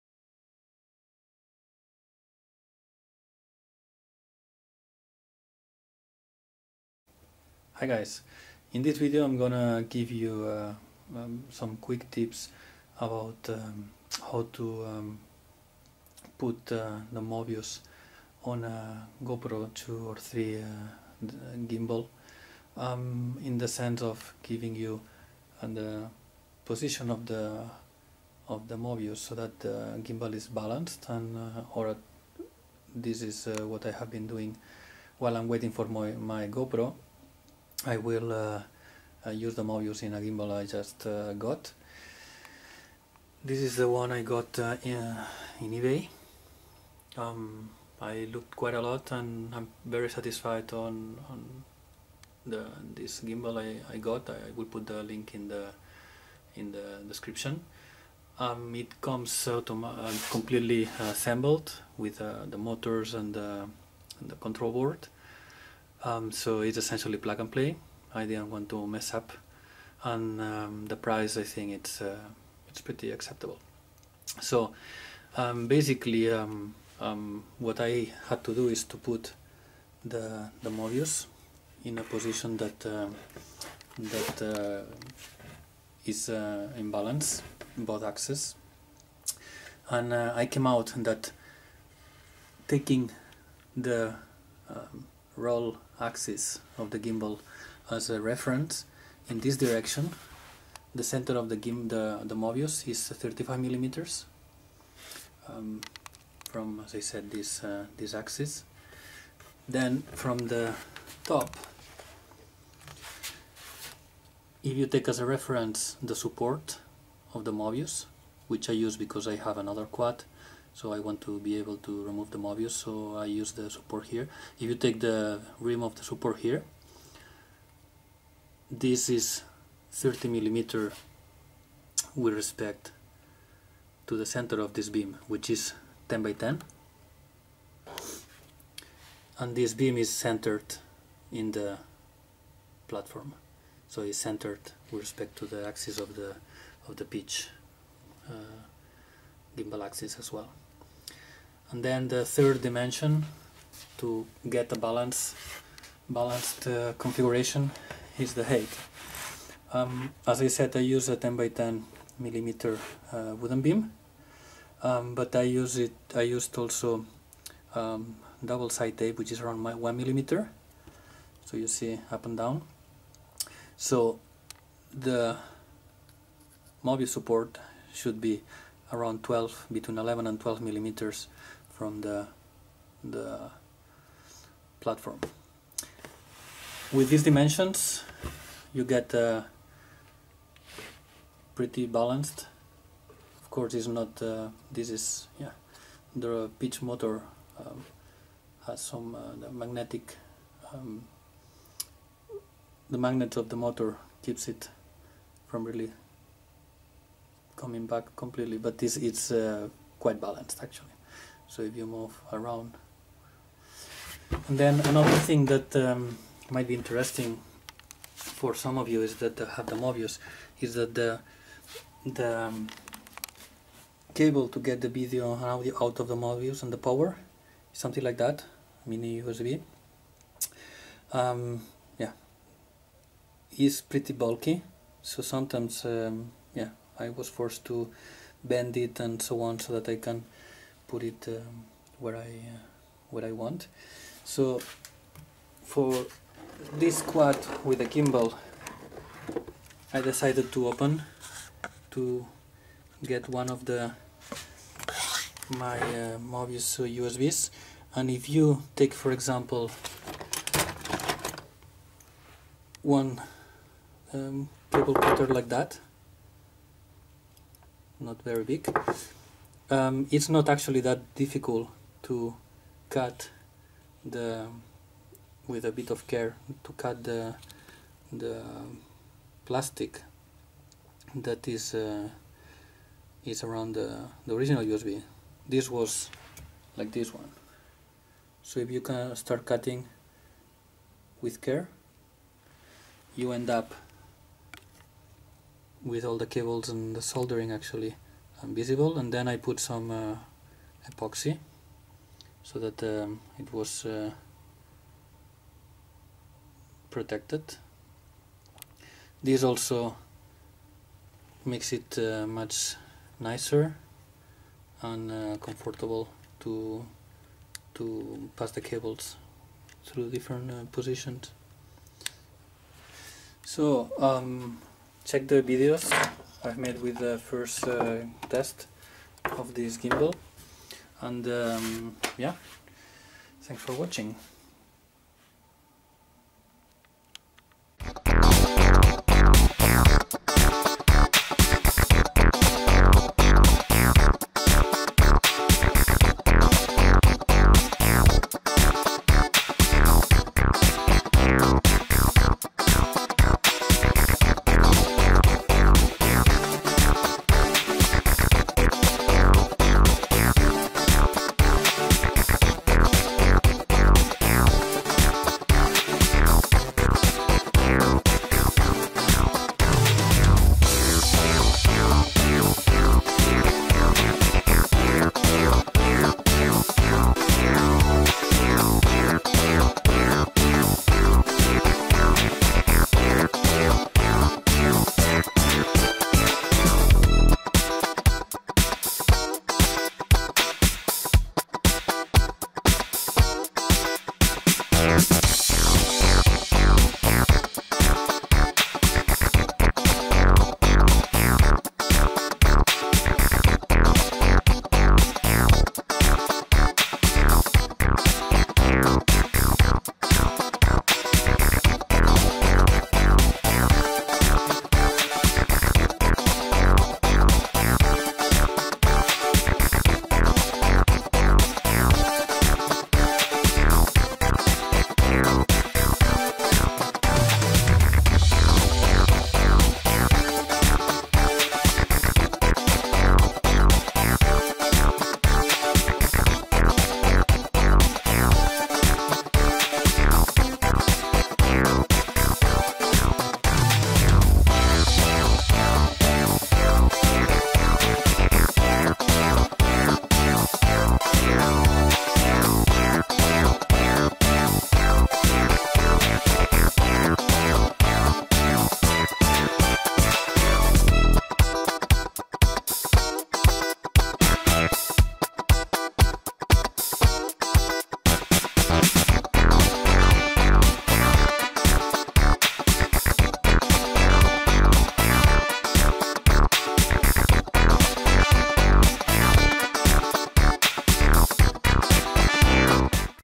How to mount a Mobius in a GoPro 2/3 gimbal
For those owning a Mobius that want to use it in a GoPro 2/3 gimbal: while waiting for my GoPro I couldn't wait for mounting and using the 2 axis brushless gimbalc thus I decided to mount my Mobius and give it a try.

Check the video for knowing where you should put the Mobius to get a balanced configuration.